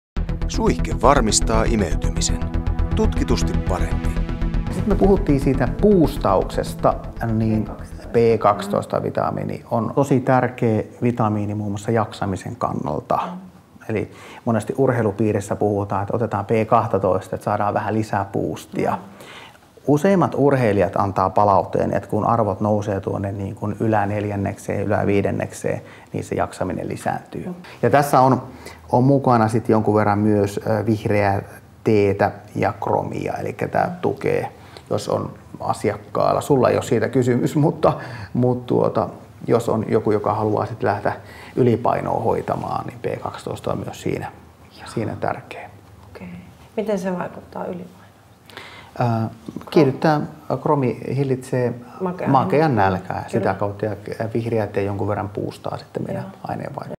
Nordic Health Sprays - Karoliina Blackburn ja vitamiinitulokset
Koe Karoliinan Blackburnin ja Jani Sompin kanssa, kuinka ratkaisu löytyy helposti yleisimpiin vitamiinin puutostiloihin.